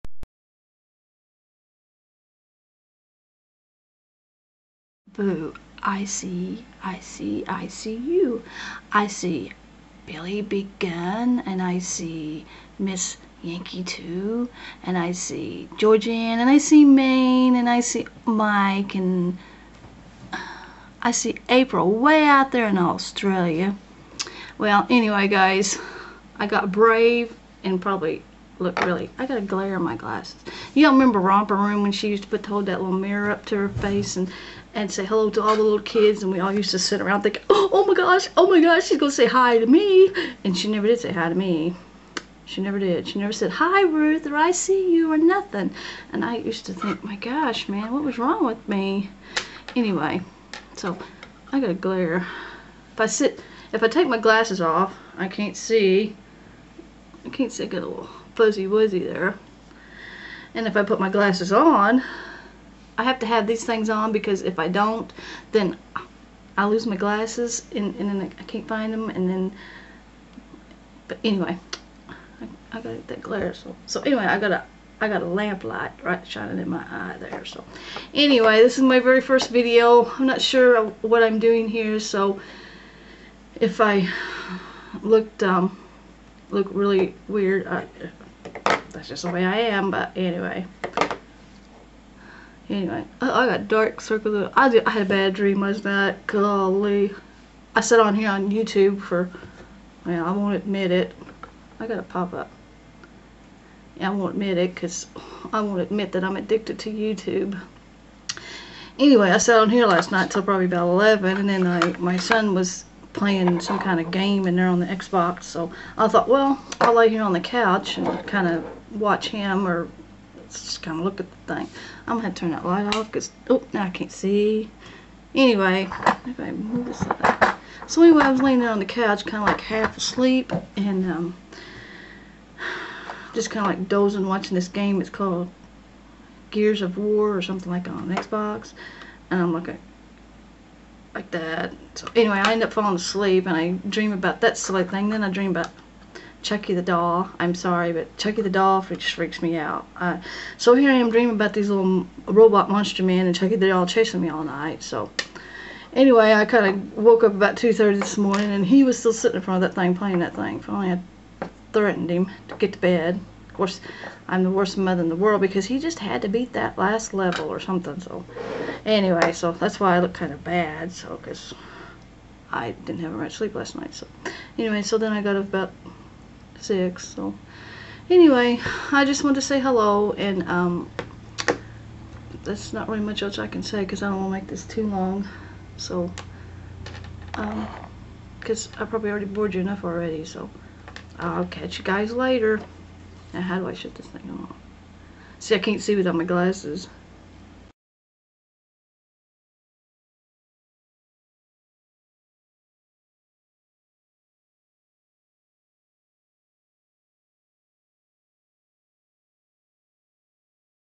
Welcome To Oshutterup's World..
(my first video - gosh, it's been that long...)
Just saying hello's and my normal everday rambling..